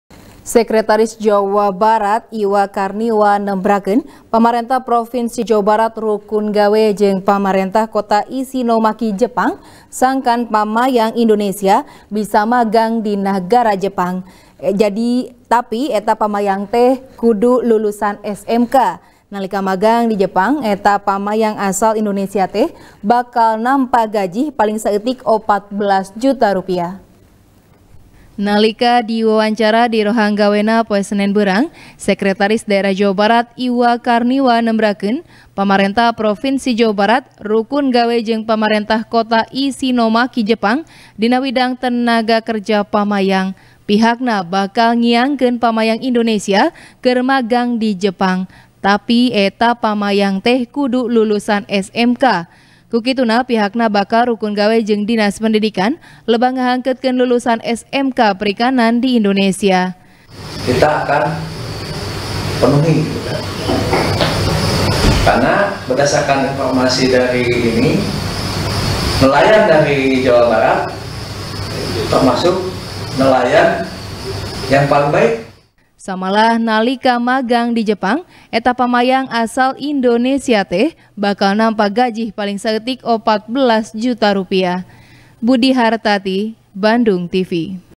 Pamayang lulusan SMK bakal magang di Jepang | TP | BANDUNGTV
TP | BANDUNGTV | ELBY NOPITRI | Nalika diwawancara di rohang gawena, poe senen beurang, sekretaris daerah Jawa Barat, Iwa Karniwa nembrakeun, pamaretah provinsi Jabar rukun gawe jeung pamarentah kota ishinomaki Jepang dina widang tenaga kerja pamayang. Pihakna bakal ngiangkeun pamayang Indonesia keur magang di Jepang. Tapi, eta pamayang teh kudu lulusan SMK. Ku kituna, pihakna bakal rukun gawe jeung dinas pendidikan lebah ngahangkeutkeun lulusan SMK perikanan di Indonesia.